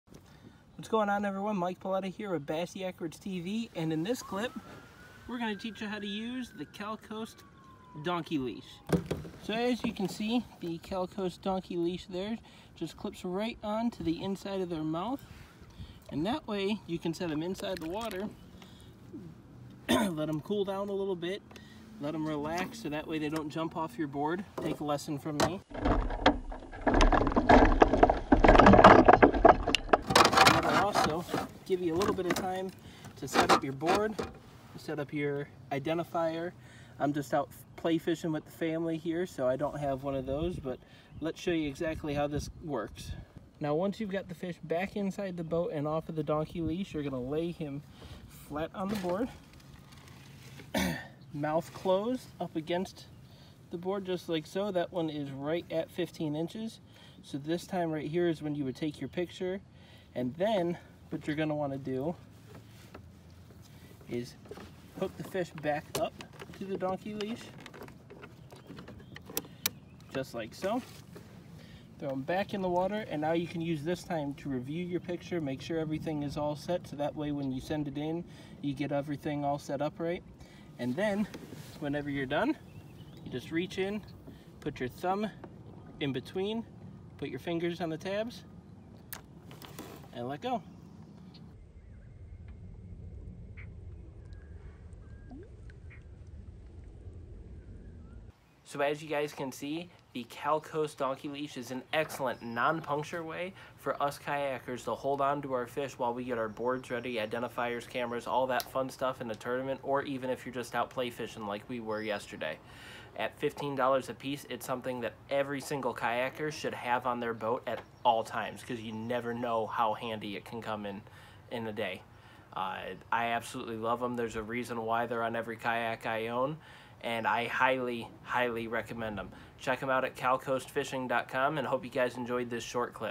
Using The Cal Coast Fishing Donkey Leash
A necessity for any kayaker or jon boat user, The CCF Donkey Leash is a great way to keep your catch under control while you get everything ready for the perfect pic! It is also harmless to the fish, which helps sportsmen around the world enjoy these great catches and memories for years to come. check them out